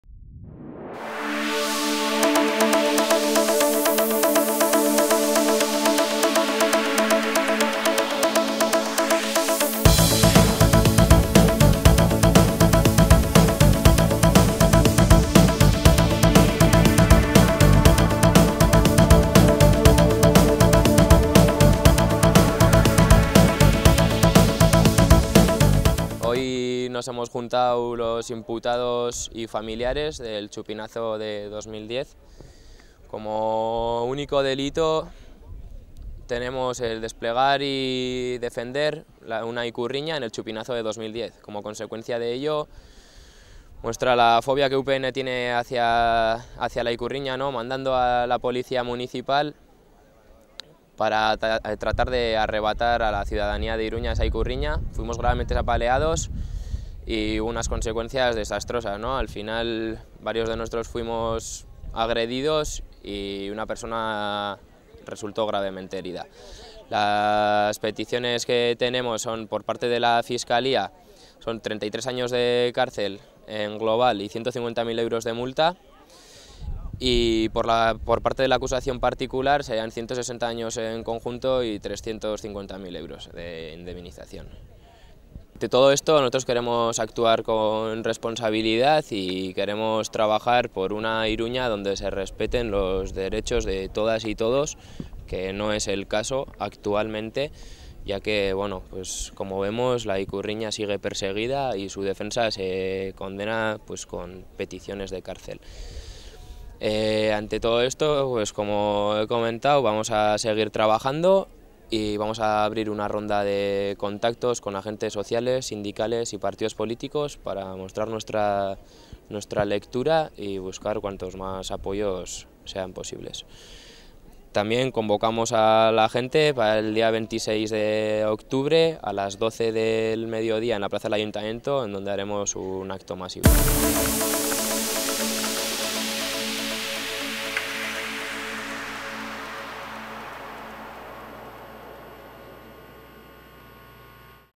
"En Iruñea se nos impone un estado de excepción en toda regla"
Personas imputadas por su presunta participación en el intento de exhibir una ikurriña en el Txupinazo del 2010 y para quienes se solicitan altas penas de cárcel, han denunciado la "fobia de UPN" contra la ikurriña y la utilización de la policía municipal para reprimir a quienes la defienden "creando verdaderos momentos de pánico y tensión con consecuencias gravísimas" en el inicio de las fiestas de San Fermin.